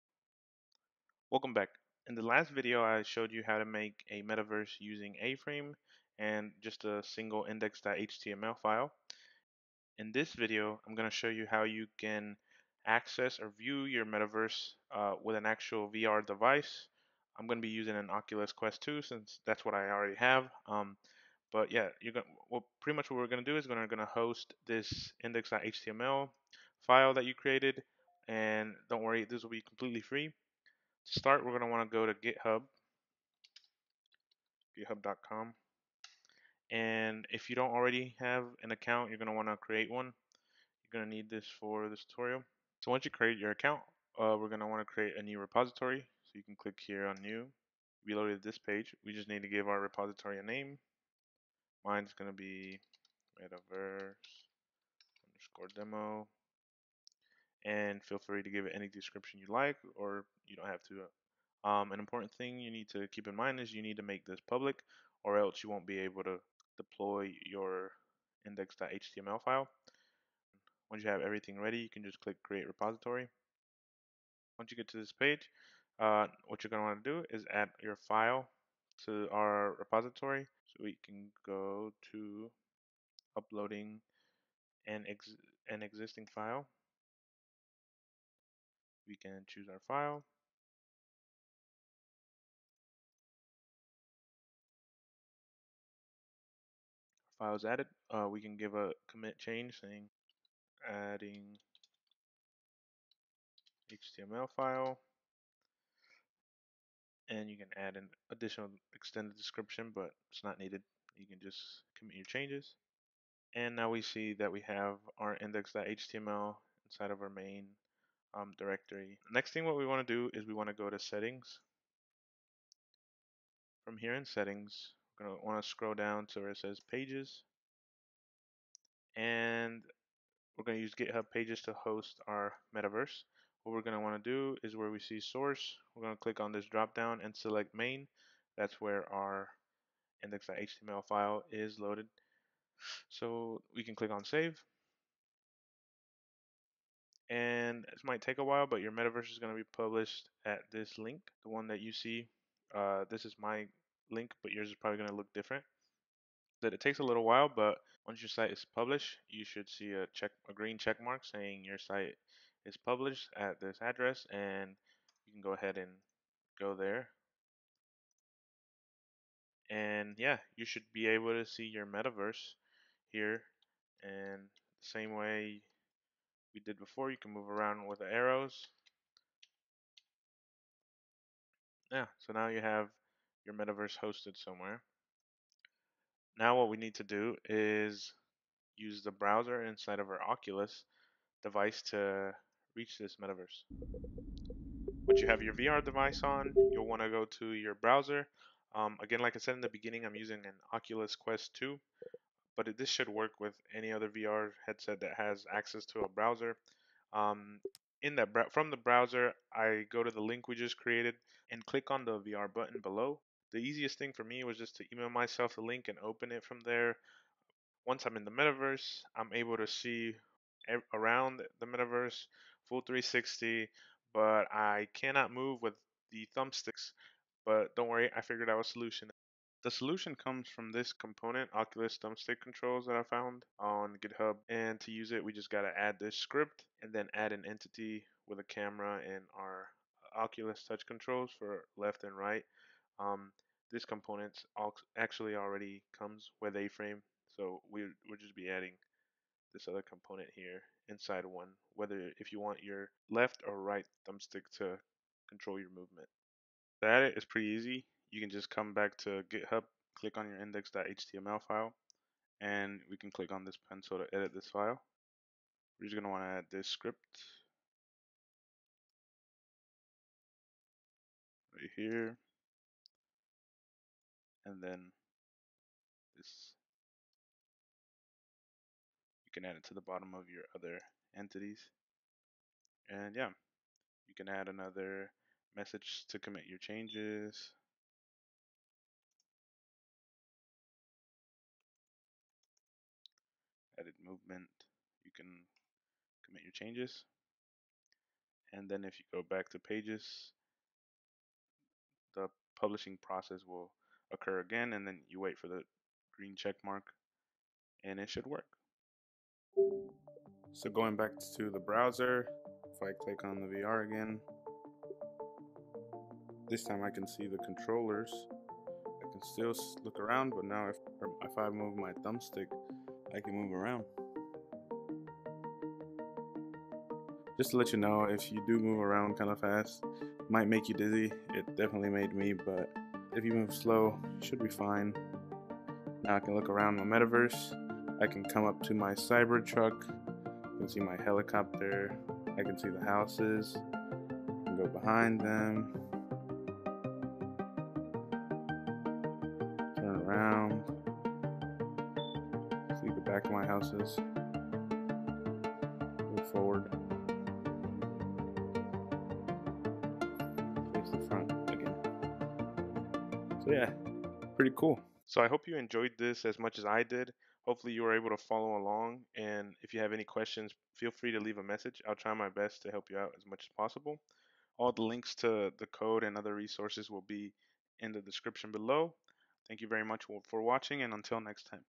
The Easiest Way to Build a Metaverse #2 - Viewing on VR Device
In this video I show you how to view the Metaverse we created in on an actual VR device